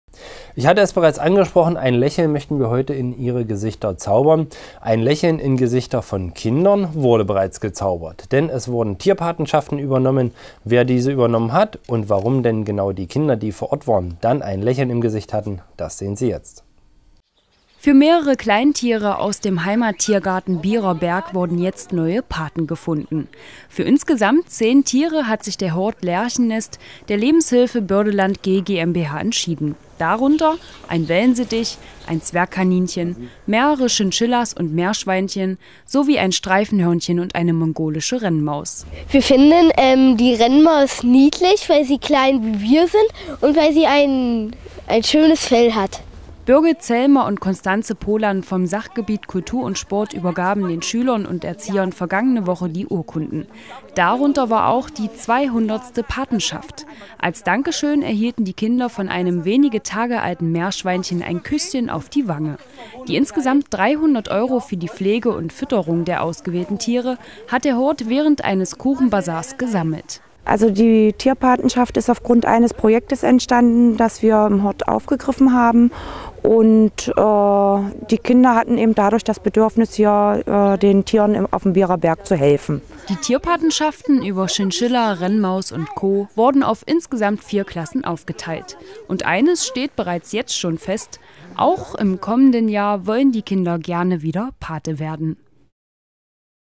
Hort "Lerchennest" übernimmt Patenschaft für Chinchilla, Rennmaus & Co
Der Hort "Lerchennest" der Lebenshilfe Bördeland gGmbH hat Anfang Februar die Patenschaft für mehrere Kleintiere auf dem Bierer Berg übernommen. Als Dankeschön gab es ein Küsschen von einem wenige Tage alten Meerschweinchen und Urkunden.